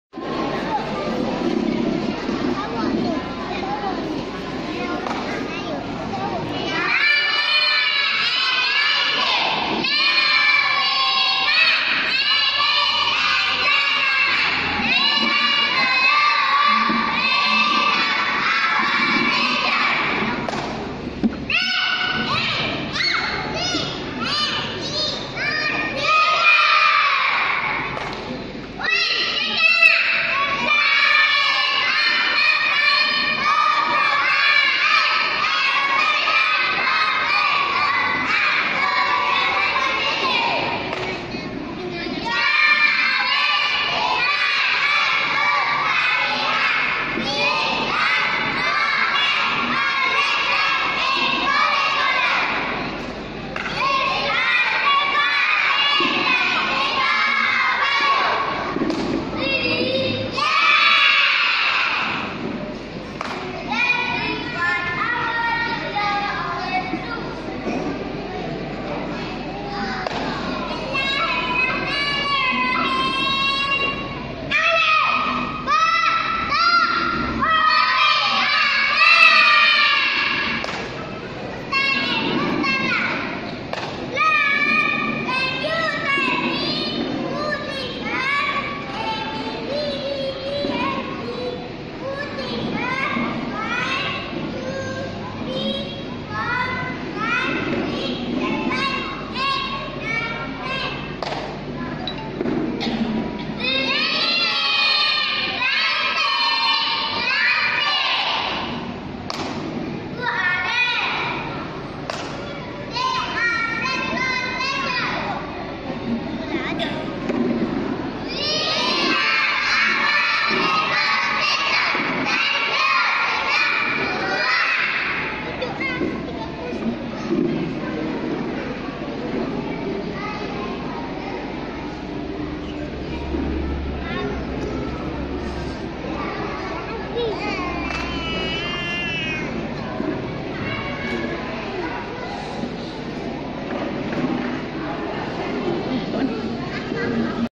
Coral Speaking Pra Sekolah Sri Cempaka SK Machang 1 (2018)
Kenangan pengalaman Arsyad join coral speaking masa pra 6 tahun.

Nanti besar dia boleh tengok sini.